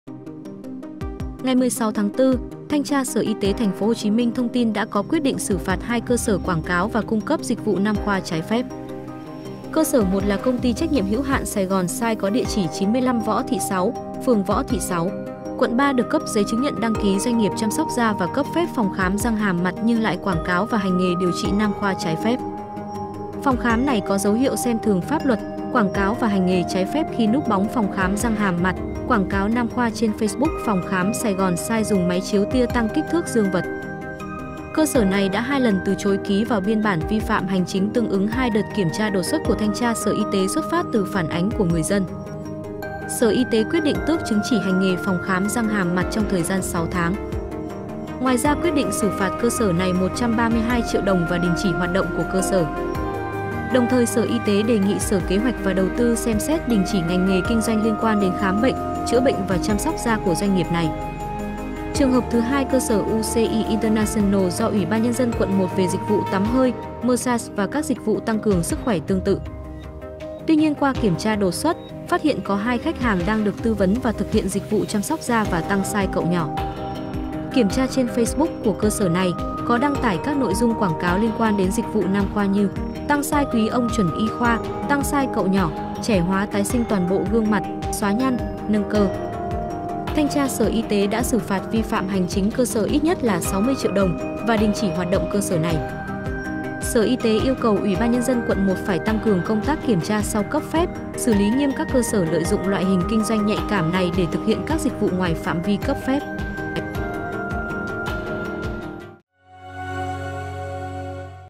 TP.HCM: Cơ sở núp bóng phòng khám răng hàm mặt, tăng 'size cậu nhỏ' trái phép
Thanh tra Sở Y tế TP.HCM vừa có quyết định xử phạt hai phòng khám quảng cáo và cung cấp dịch vụ tăng 'size cậu nhỏ' trái phép.